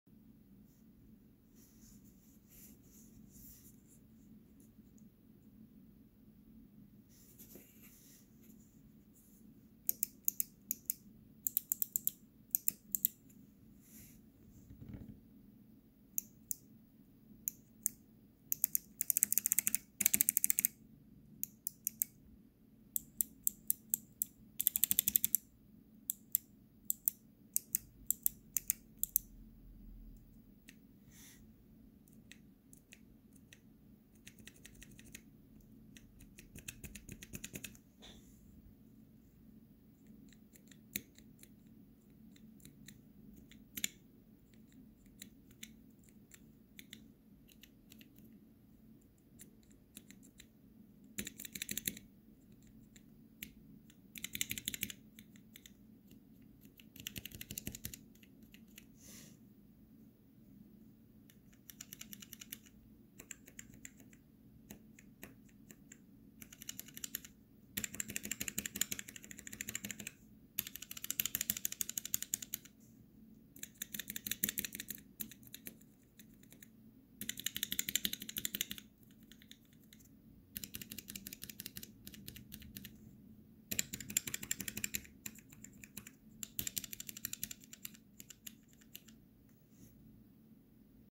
Kailh Chocs sound test: switch sampler from MKUltra
Used a Kailh box switch sampler frame to demo these, so YMMV on the sound based on the material of the switch plate and the keycaps.

Top row (left to right):
White (clicky) - 60g
Robin (clicky) - 57g
Brown (tactile) - 60g

Middle row (left to right):
Black (linear) - 60g
Red (linear) - 50g
Silver (linear) - 40g

Bottom row (left to right):
Pro Red (linear) - 35g
gChoc (linear) - 20g

I wouldn't recommend the tactiles, they don't feel great. Otherwise, I'd recommend picking up a sampler like I did to feel the difference, it's more important to me than the sound.